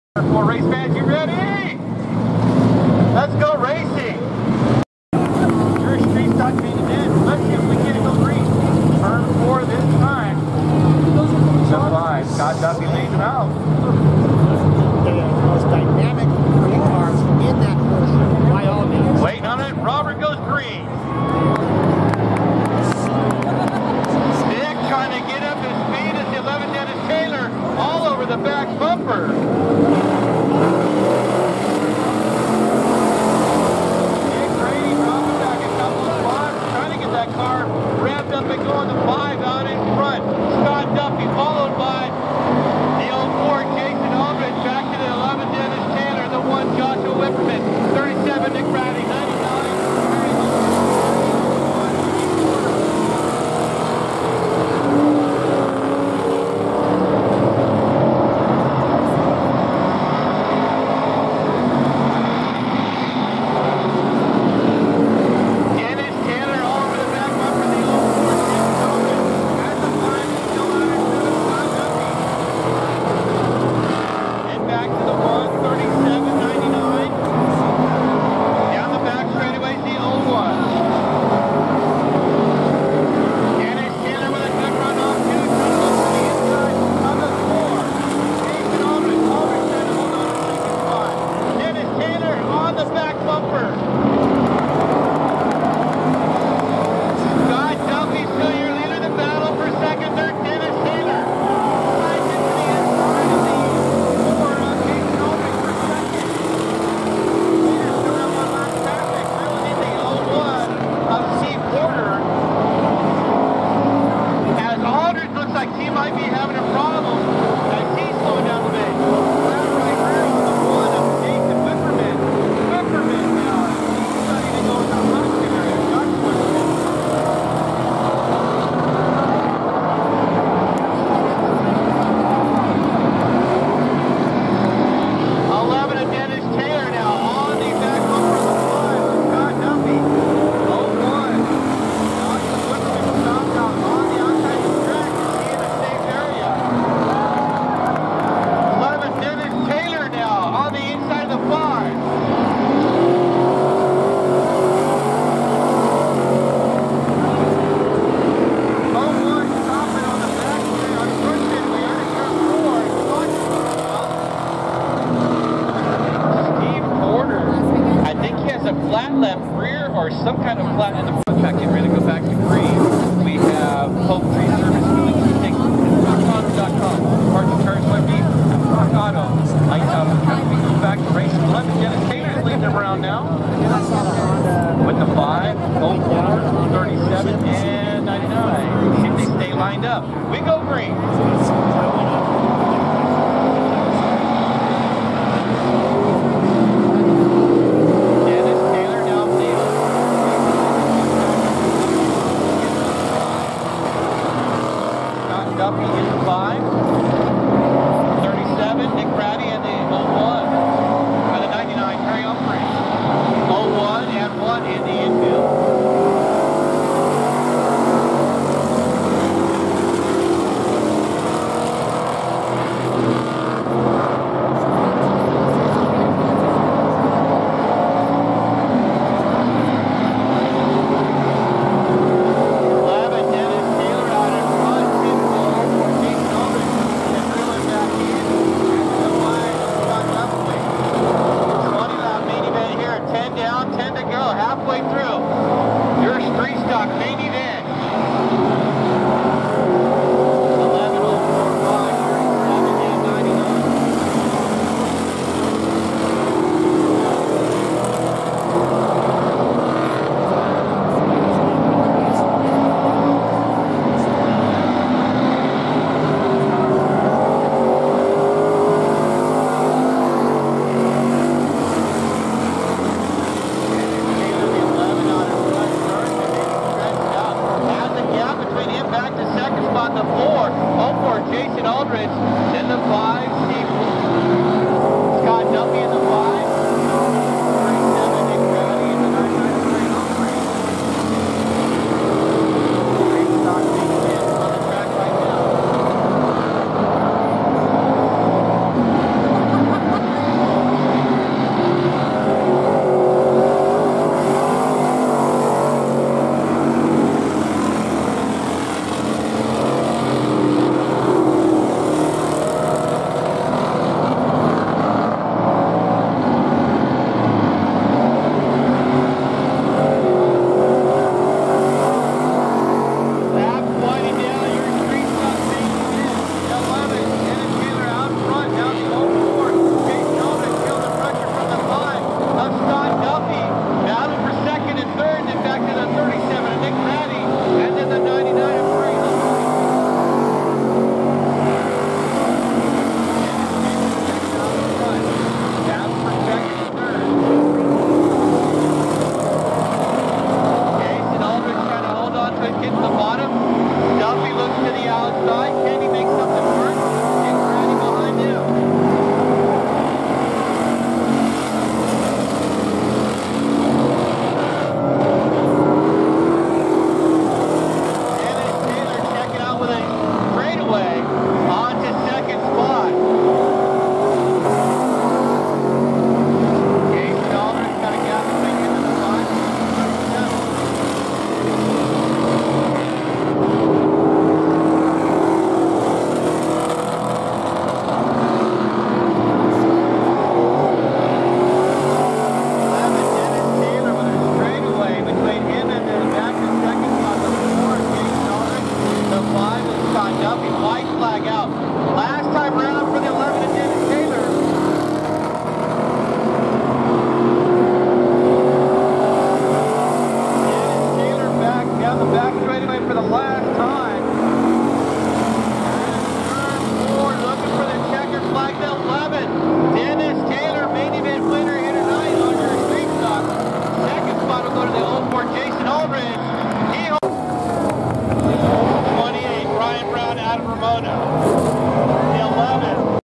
Street stock main Barona Speedway 10-16-2021
STREET STOCKS
A FEATURE 1
1 5 11 Dennis Taylor Dennis Taylor San Diego, CA 4
2 3 04 Jayson Aldridge Jayson Aldridge Lakeside, CA 1
3 1 5 Scott Duffy Scott Duffy Rancho Bernado, CA -2
4 2 37 Nick Braddy Nick Braddy El Cajon, CA -2
5 7 99 Perry Humphries Perry Humphries San Diego, CA 2
6 DNF 8 01 Steve Porter Steve Porter Campo, CA 2
7 DNF 6 1 jashua wipperman jashua wipperman San Diego, CA -1
DNS 4 28 Kaden McKown Kaden McKown Ramona, CA -